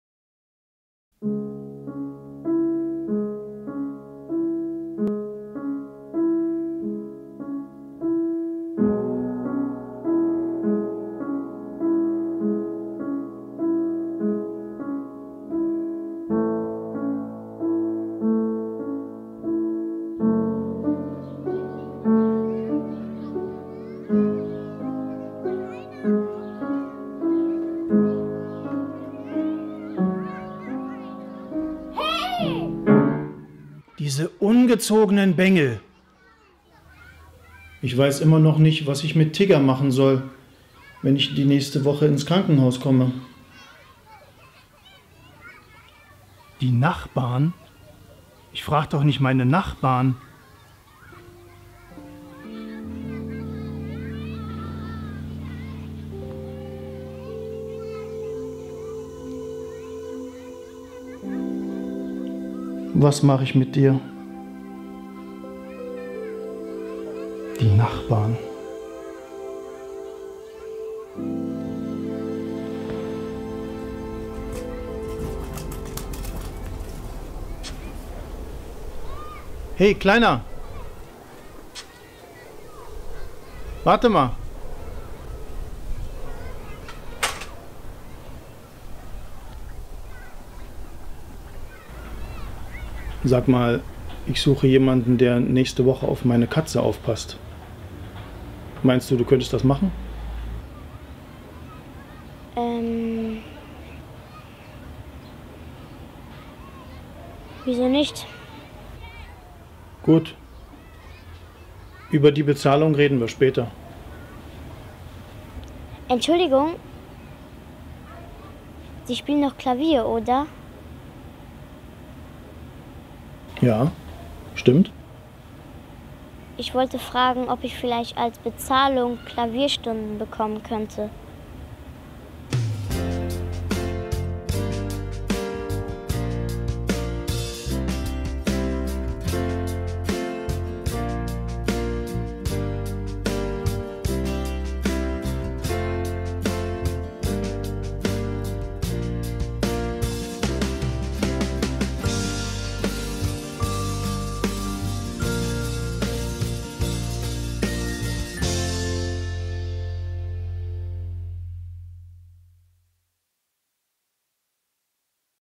Umdenken
Mein Beitrag zum Videowettbewerb "Gesunde Nachbarschaften" der AOK Rheinland/Hamburg.

Update: Der Film hat bei o.g. Wettbewerb den 3. Preis gewonnen.